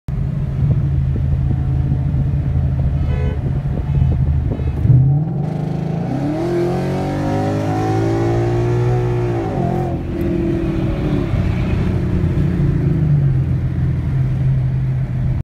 My 3.7 Would have smoked him....
Sorry about the shitty camera he caught me off guard and i didn't have my gopro setup.

This is exactly why i wish i never traded in my 3.7 manual for a auto 5.0... Wheres the fun in losing to a Caliber SRT4 ??